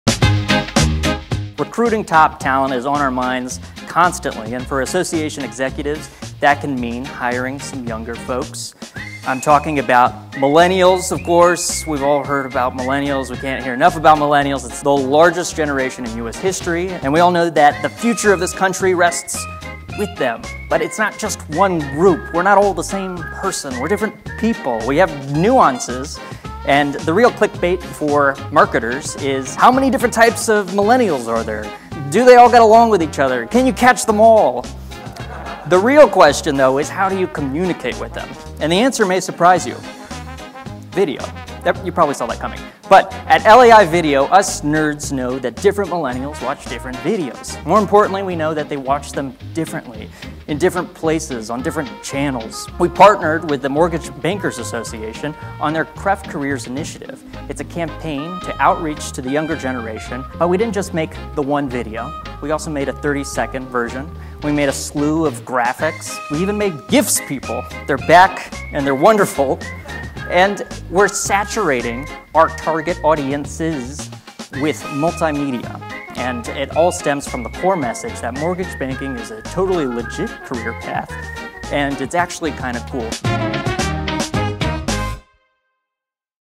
Targeting the Right "Millennials" for Your Industry
Ever since the term “millennials” was coined, we could not escape the non-stop discussion about the generation. While organizations and industries seek to attain this mysterious demographic, marketers are only recently confirming that millennials are not, spoiler alert, all the same.